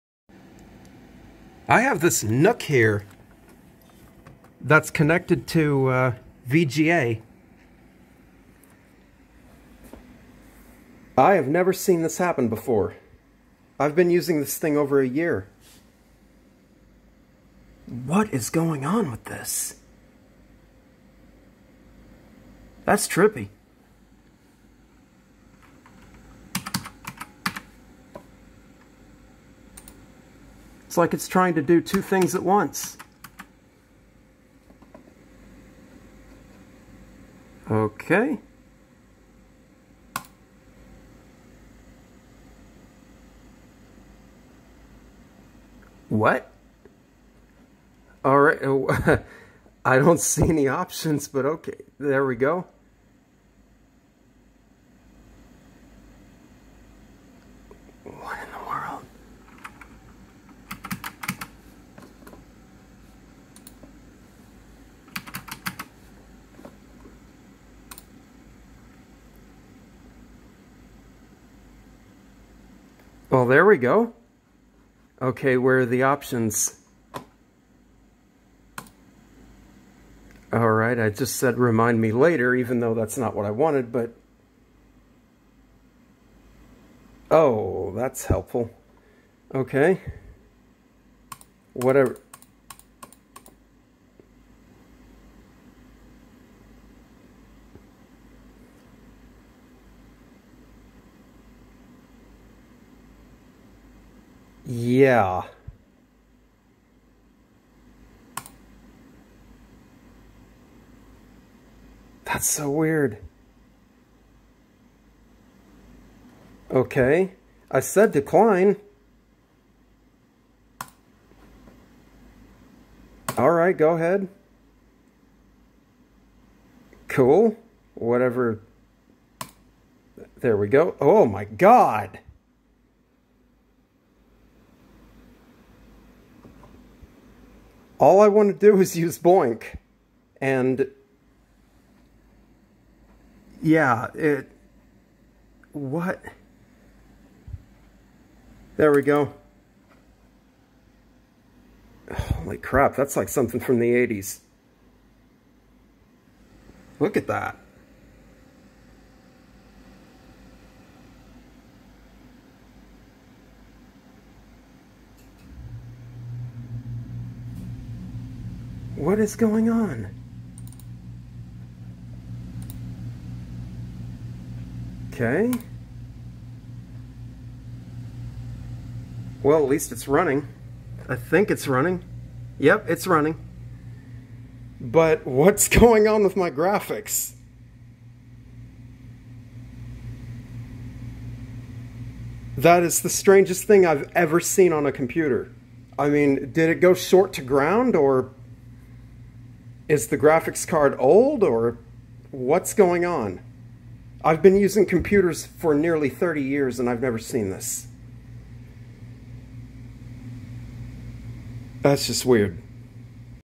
The Weirdest Glitch I Have EVER Seen On A Computer! (EPILEPSY WARNING)
The camera's autofocus didn't like this too much due to extreme glitchiness.

I was trying to resume running BOINC World Community Grid on a tiny computer when suddenly it started to glitch out. This was one of the strangest things I've ever seen in all of my years of computing. The computer works, but at the same time it doesn't.

35 minutes later: The GPU is now being utilized by a BOINC OPNG work unit and it's working faultlessly, yet the graphics are still glitching.

UPDATE: About 6 hours after this happened, I recorded a new video with the intent of showing more glitches only to find that the computer was working perfectly fine, and it continues to work properly. This node/NUC is from 2015, so it's over 7 years old as of this recording. From my experience, computers usually work faultlessly after 7 years, but this is miniature technology from last decade, so perhaps it's not as robust as a laptop or a desktop. With that said, this was still a strange occurrence. What causes these problems?